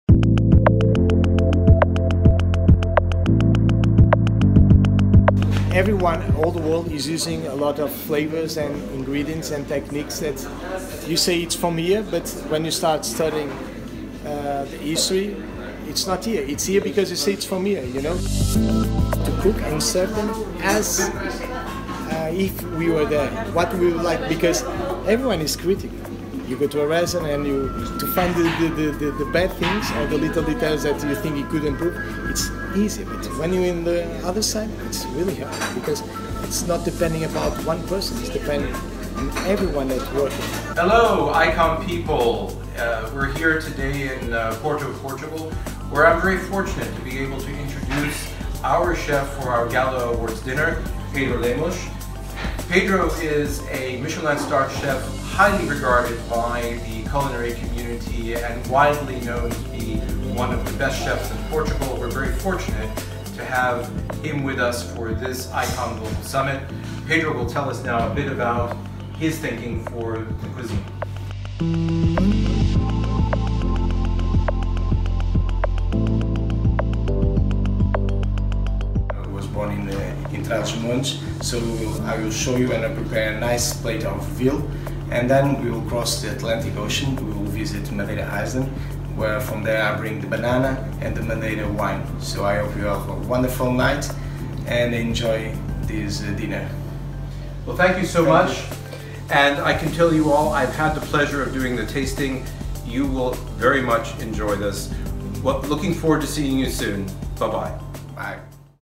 I-COM Global Summit 2017 - Gala Awards Dinner Chef
Video introducing the Chef of the I-COM Gala Awards Dinner - Michelin star chef Pedro Lemos